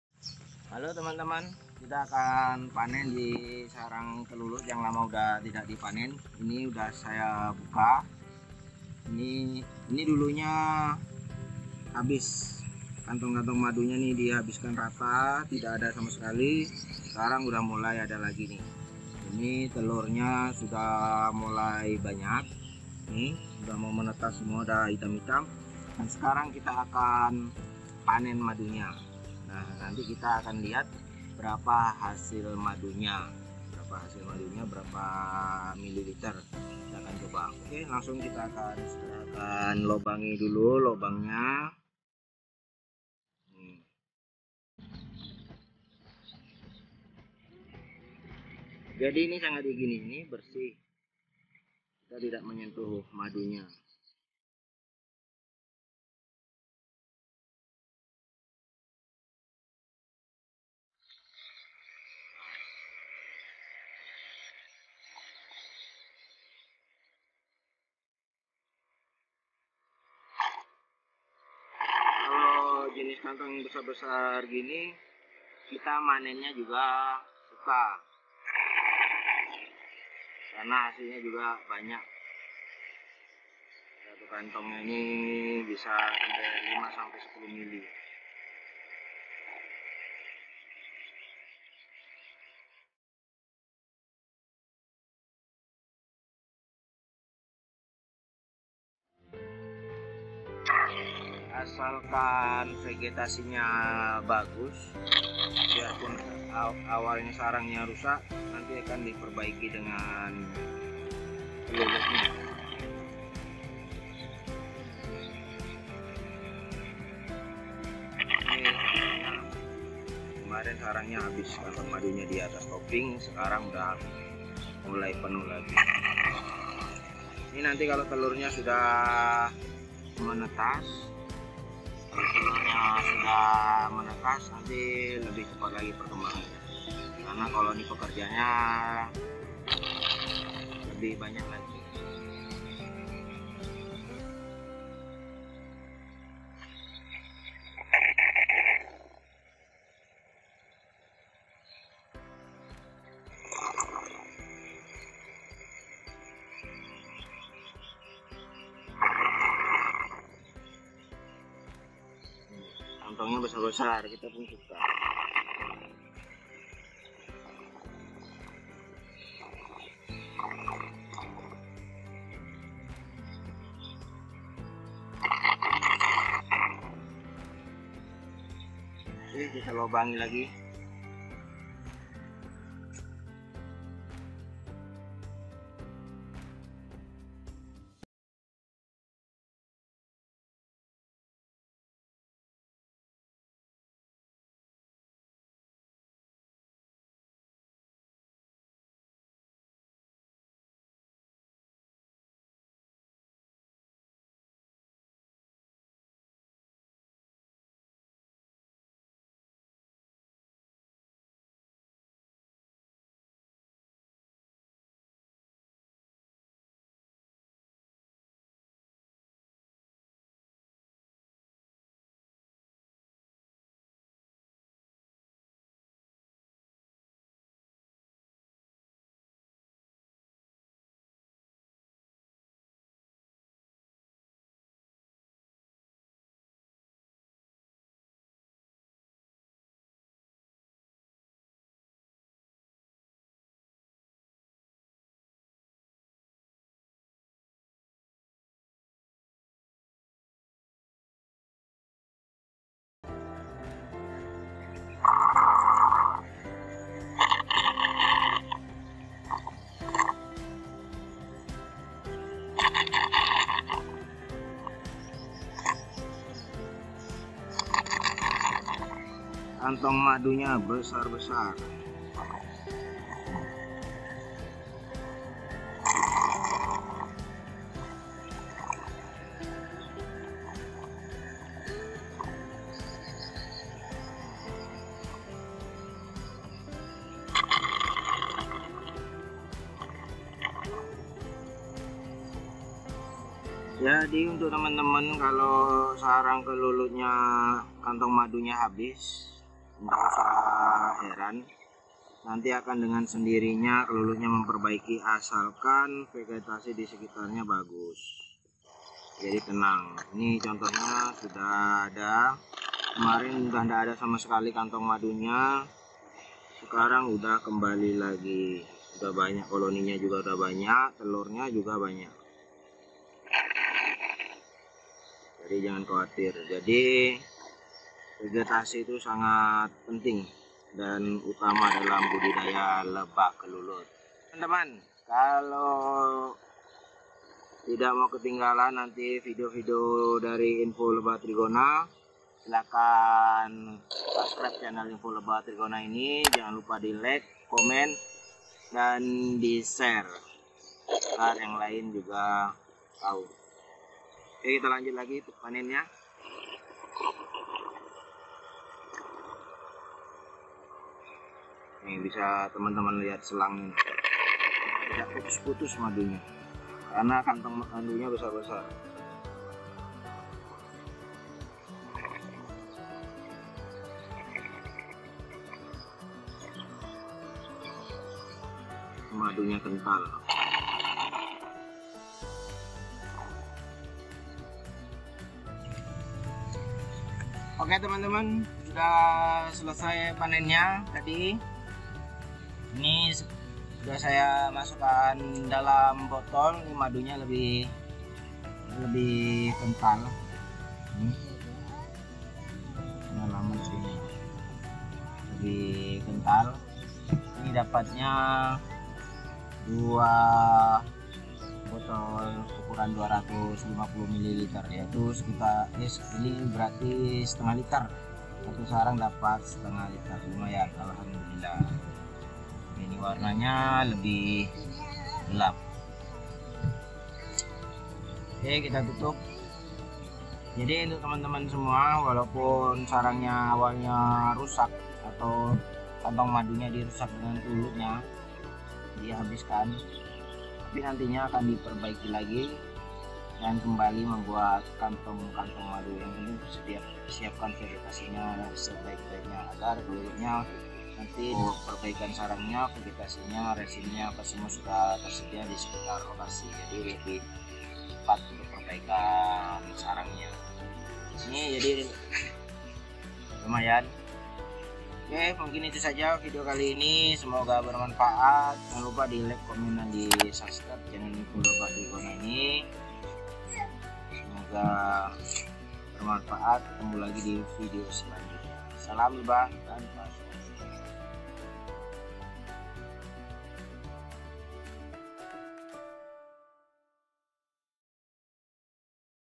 SARANG KELULUT RUSAK - SEKARANG DI LUAR DUGAAN!
Sarang Kelulut rusak sudah produktif kembali, ini srang sudah lama kantong madunya di ratakan oleh lebah kelulut dan habis yang ada di dalam topping.

Tapi sekarang sudah normal kembali sudah banyak kantong madu dan telur juga sudah naik topping, telur ini kalau menetas akan menambah koloni pekerja sehingga produksi madu kelulut makin cepat.

Support terus Channel @madutama agar makin berkembang dan bermanfaat bagi seluruh peternak lebah kelulut.